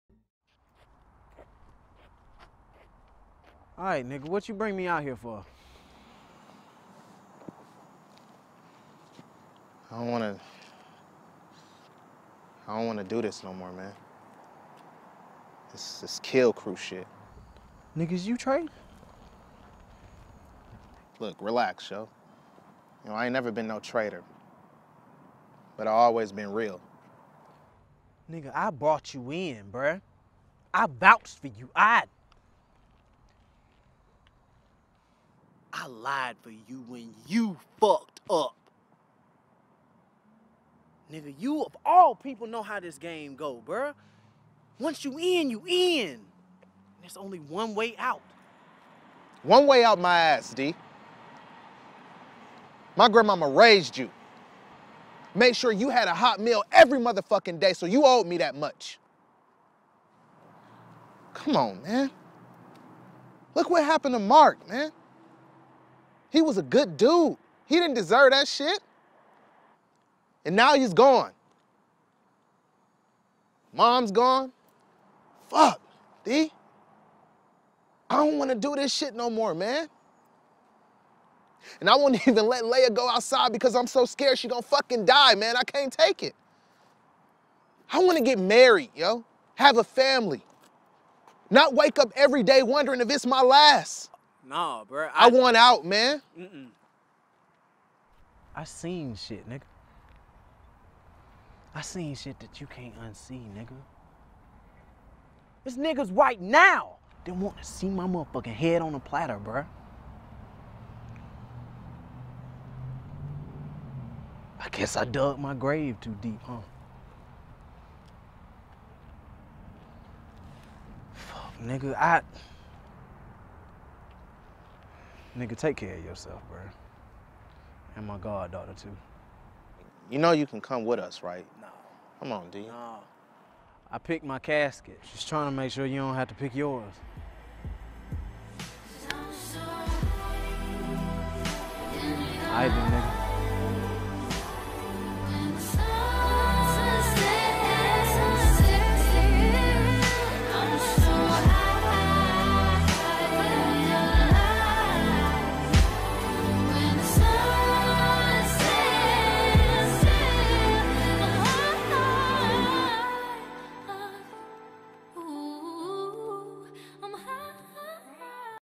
I want Out
Mad at my boy for trying to duck his responsibility